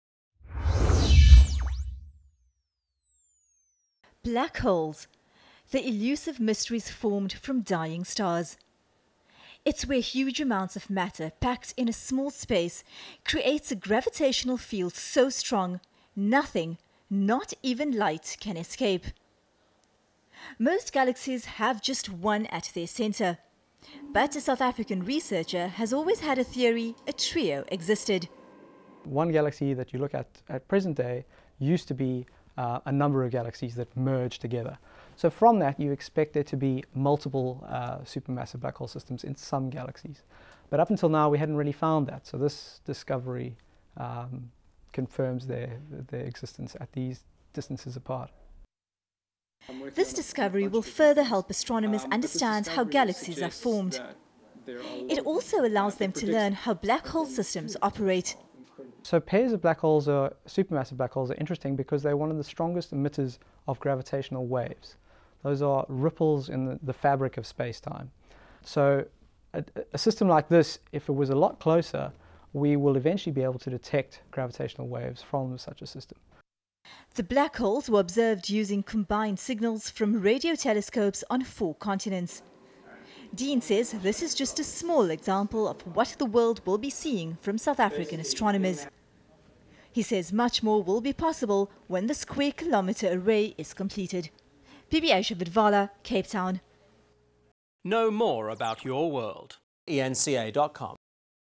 SA researchers discover trio of supermassive blackholes
Cape Town - A major astronomical breakthrough has been discovered billions of light years away by a team of young scientists. The three super-massive black holes were spotted by South African researchers at the Square Kilometre Array project.